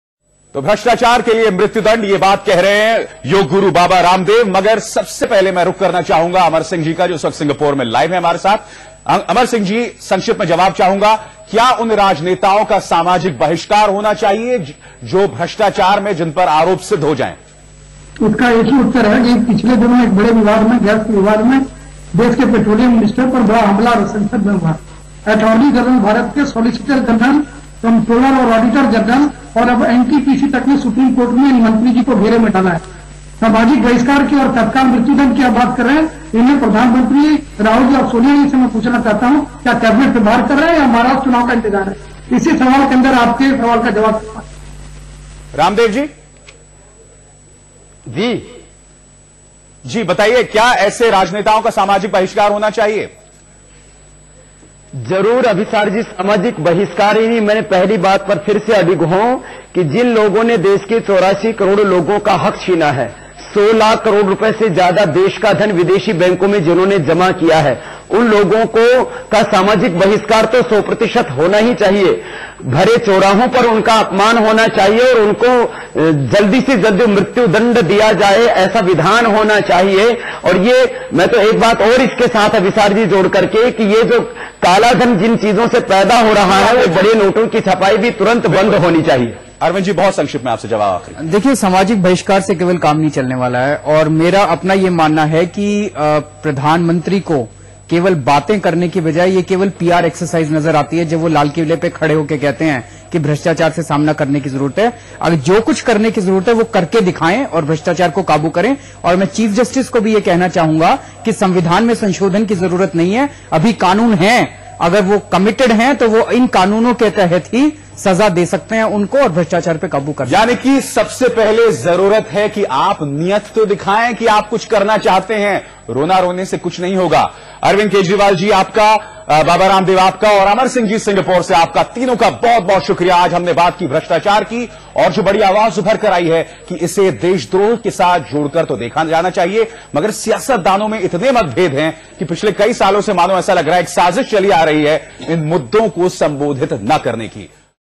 When will corruption stop in India? 4 of 4
Corruption has been a serious challenge to this country. In recent times, the debate over black money deposited in Swiss bank accounts has occupied centrestage. AajTak organised a debate over corruption in India & when will it stop? The participants included Yoga Guru Baba Ramdev, Samajwadi Party leader Amar Singh and RTI activist O.P. Kejriwal.